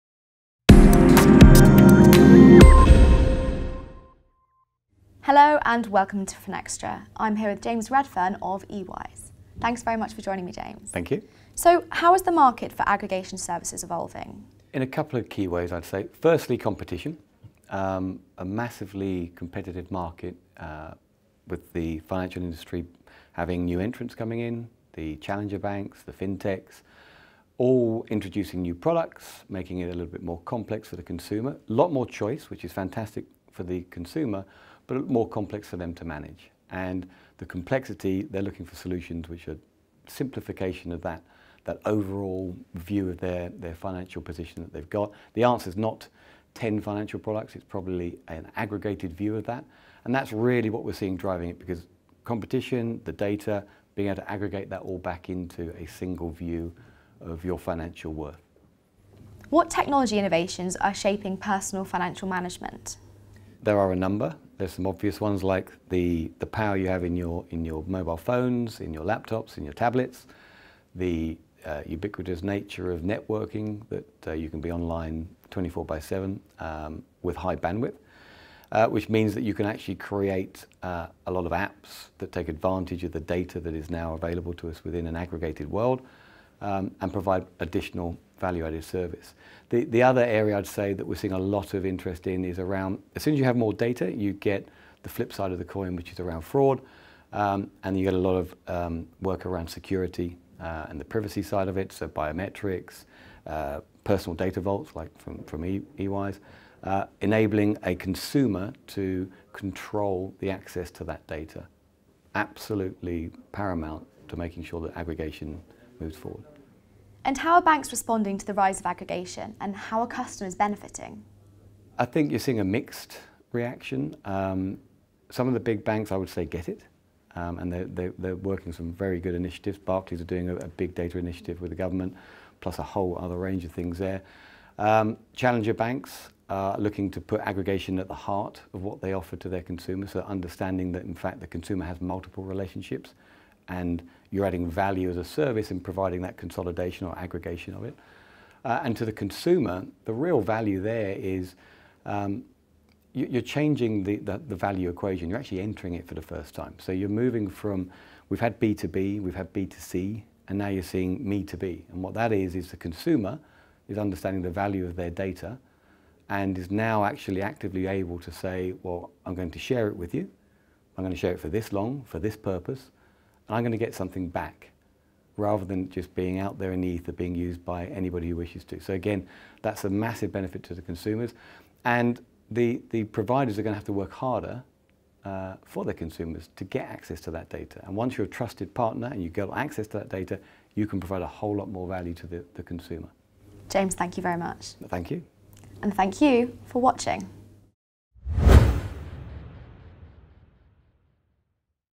Finextra: Video interview with James Redfern - about the Me2B economy
We’ve had B2B, we’ve had B2C, now it’s Me2B
James Redfern, Head of Sales, eWise, speaks about how the market for aggregation services is evolving with technology innovations, and banks’ responses to the rise of aggregation and how it benefits customers.